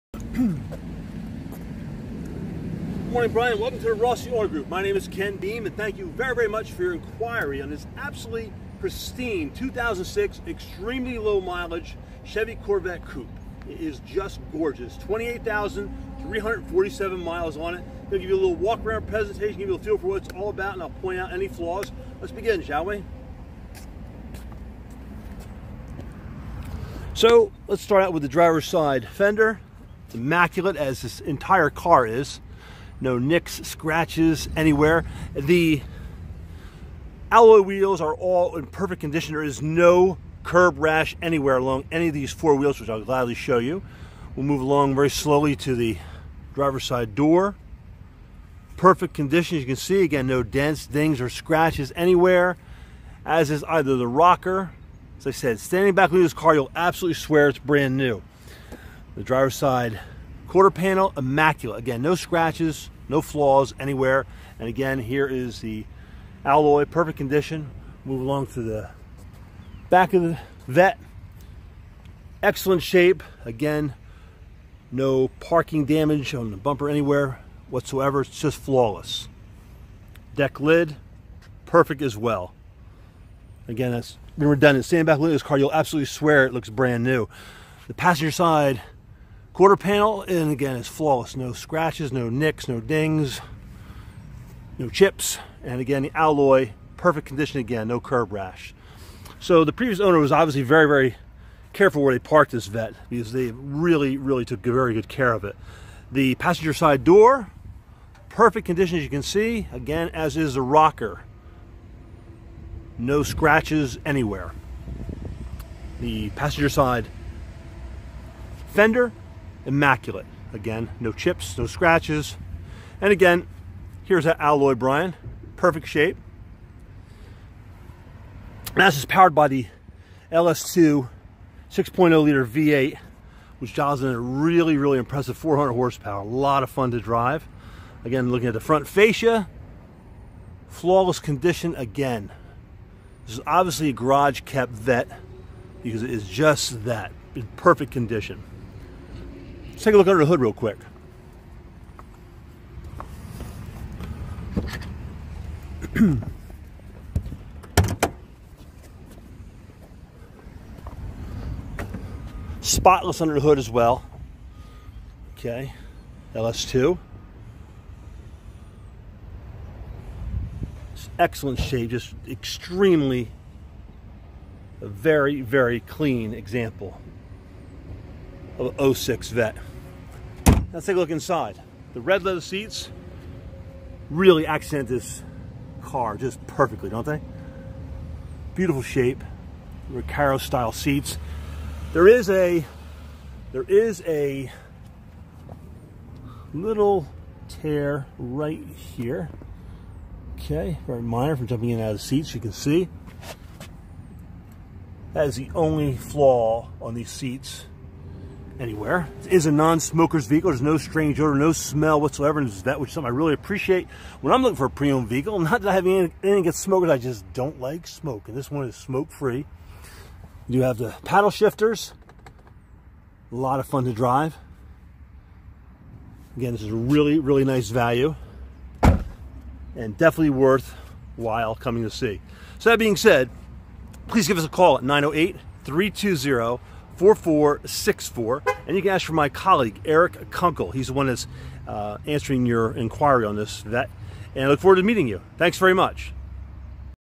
2006 Corvette Coupe at the Rossi Auto Group in Washington New Jersey
2006 Corvette Coupe at the Rossi Auto Group in Washington New Jersey shown by Customer Care Mgr Ken Beam